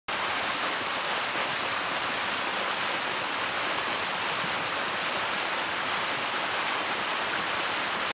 Meditative limestone waterfall
BuaTong Waterfall is a limestone creek in Mae Tang, Thailand. Limestone rocks mean you can walk up and down the falls w/o slipping - aka Sticky Falls.